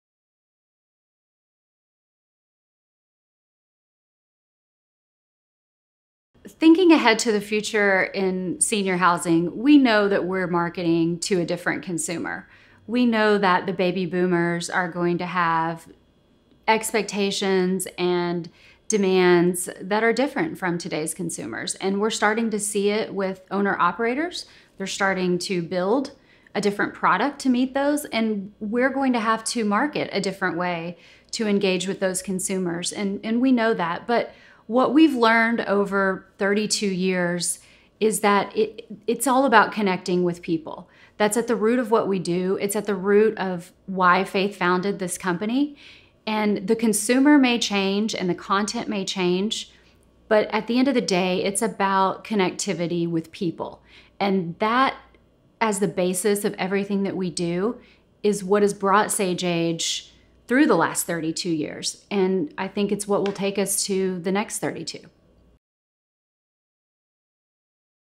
Melinda Schmitz, President of SageAge Strategies
Melinda Schmitz, our President discusses the changes in the Senior Living industry and how connectivity will keep SageAge on top.